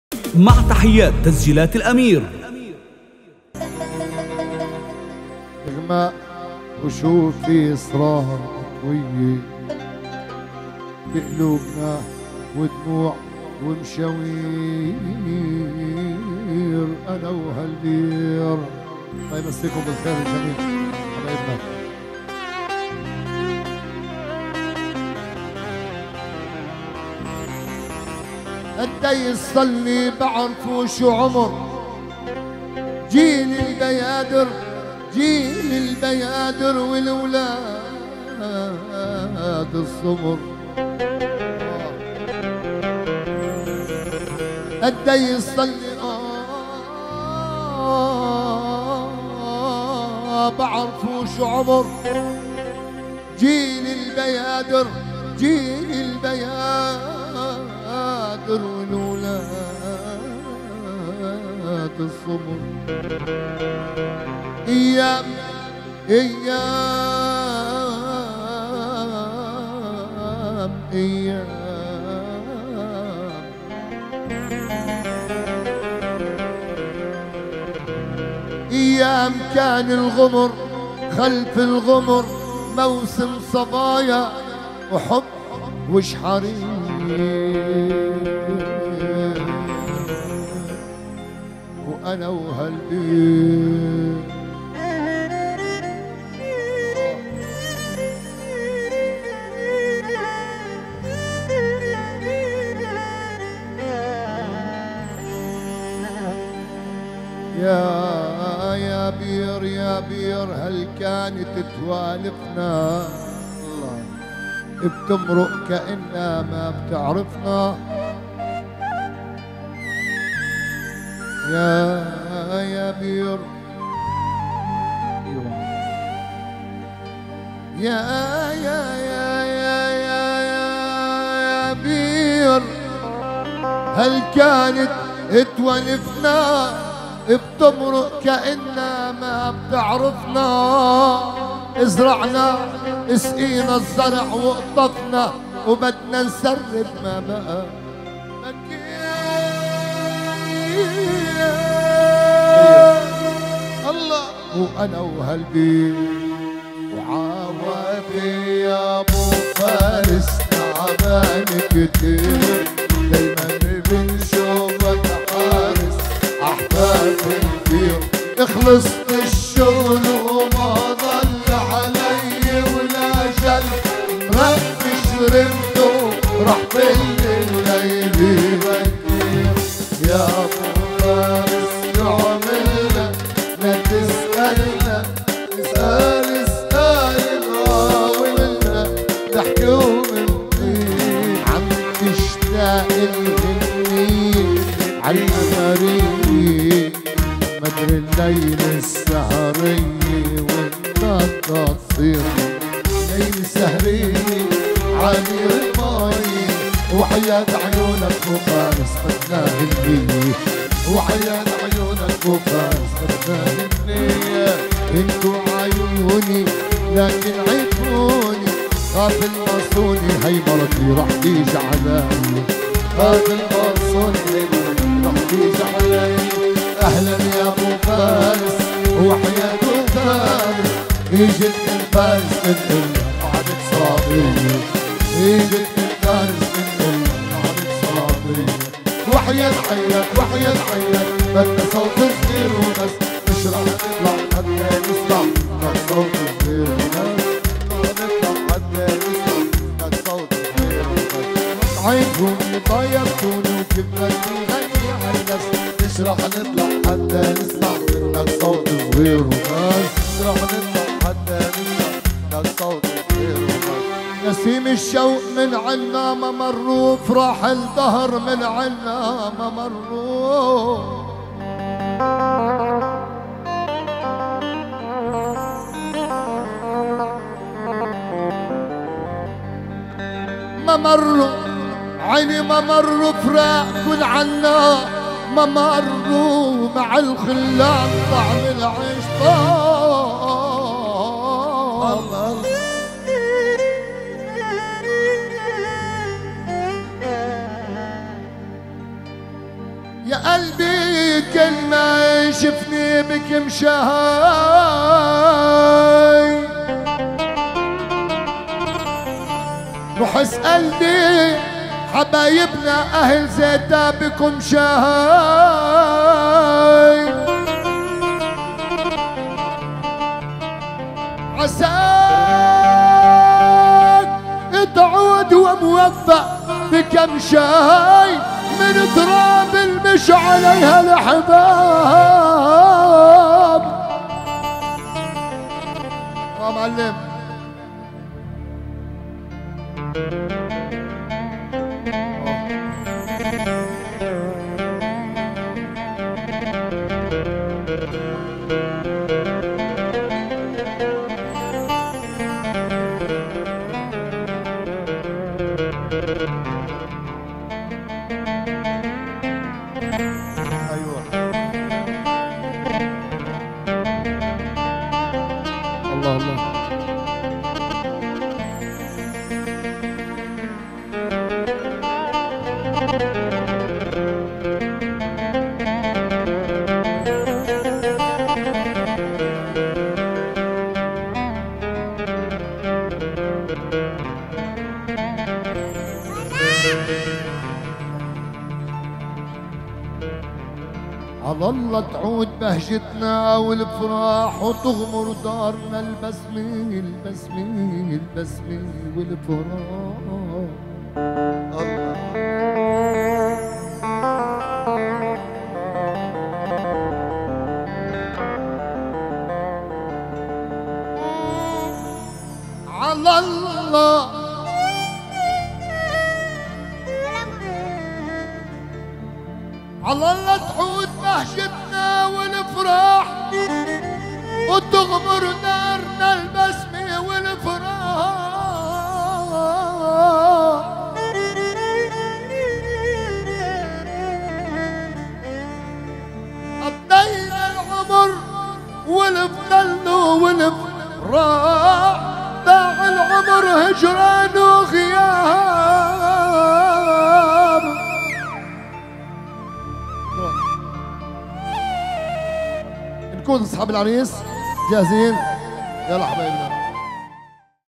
النجم احمد الكيلاني مواويل زفة العريس محمد ابو حمدة زيتا تسجيلات الأمير 2022
تسجيلات الامير
  تم فتح قناتنا الثانية لتنزيل أجمل الفيديوهات من الحفلات الجديدة
لا تنسوا الأشتراك وتفعيل زر الجرس
او زوروا صفحتنا على الفيسبوك